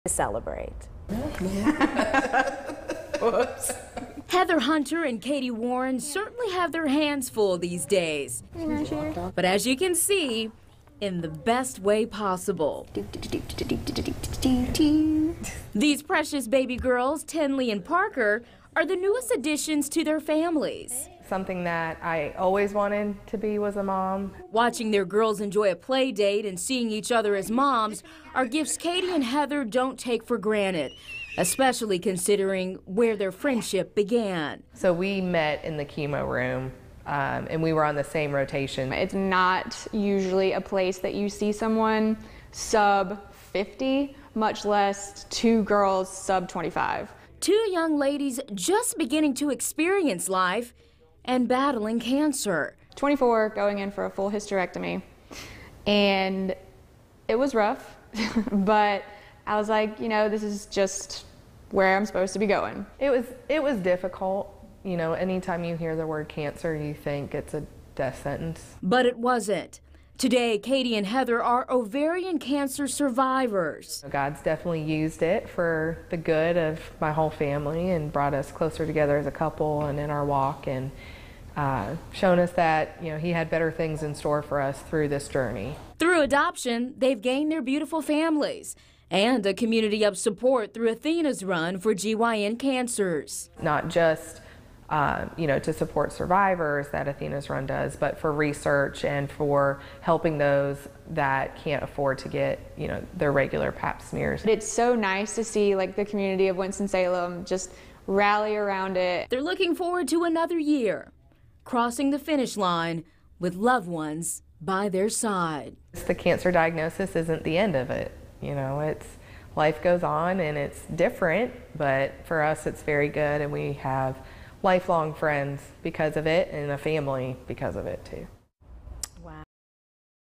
2 women who survived ovarian cancer in their 20s to honor milestones in Athena`s Run
Heather Hunter and Katie Warren share a bond that most people in their 20s never experience.